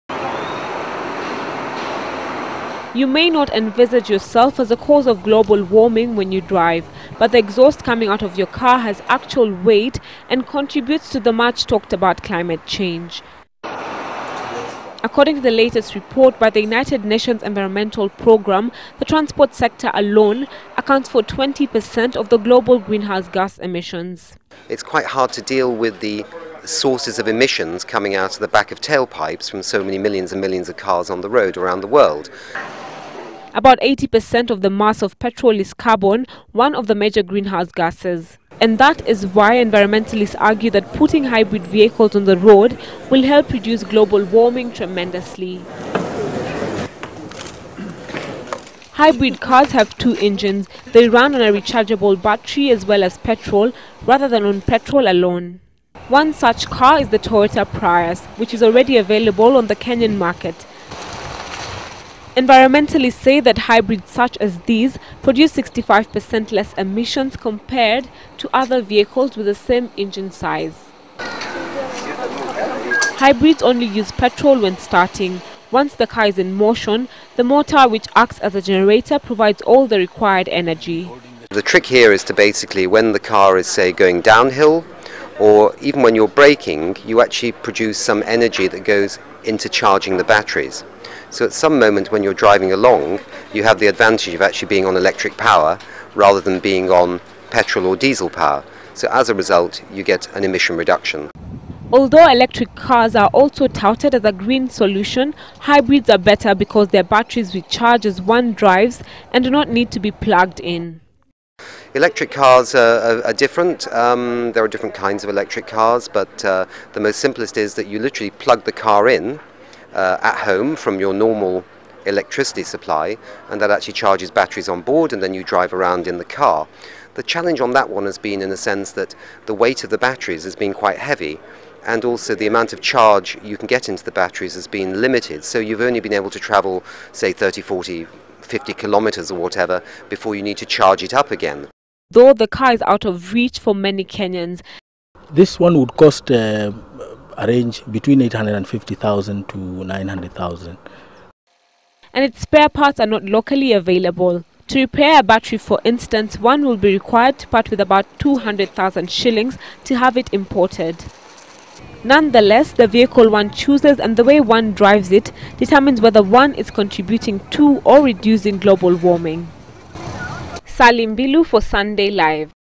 Climate Change :Hybrid Cars
With 200 nations pondering on a treaty in Copenhagen to succeed the Kyoto protocol on green house gas emissions, the transport sector has come under scrutiny with environmentalists rooting for the adoption of hybrid cars as a step towards reduction of carbon dioxide emissions. But will hybrid cars bring closer the dream of a green revolution that is envisaged in the Copenhagen deal?